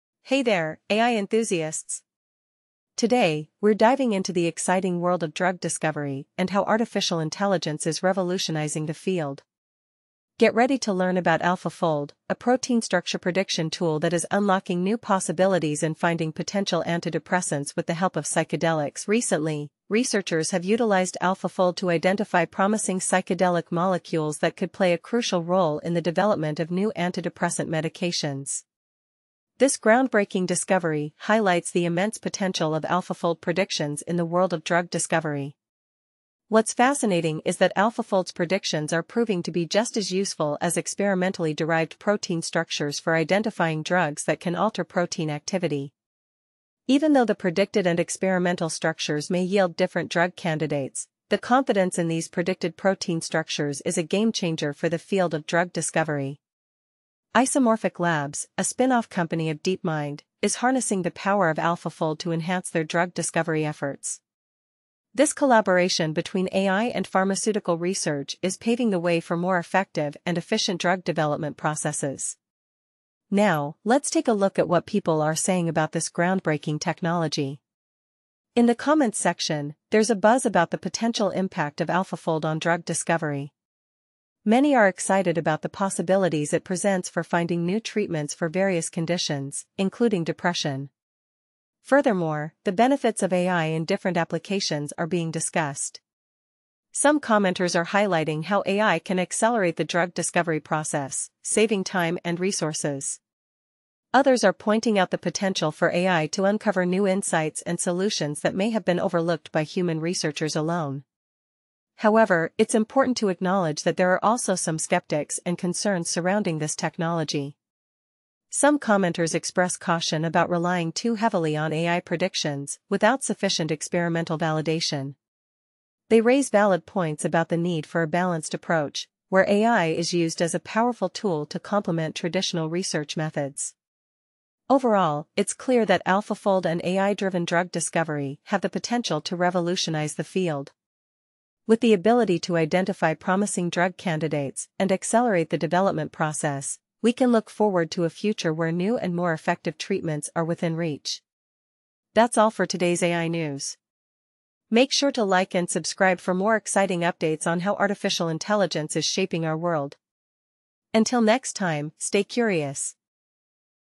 AlphaFold: Unlocking New Antidepressants with Psychedelics | AI Revolutionizes Drug Discovery
Researchers have used the protein-structure-prediction tool AlphaFold to identify potential new psychedelic molecules that could aid in the development of new antidepressants. This demonstrates that AlphaFold predictions can be as useful as experimentally derived protein structures for drug discovery. AlphaFold structures can identify drugs that alter protein activity, even though the predicted and experimental structures yield different drug candidates. Confidence in predicted protein structures could be a game-changer for drug discovery. Isomorphic Labs, a spin-off company of DeepMind, is using AlphaFold for drug discovery efforts. The comments discuss the potential impact of AlphaFold on drug discovery, the benefits of AI in various applications, and the skepticism and concerns surrounding the technology.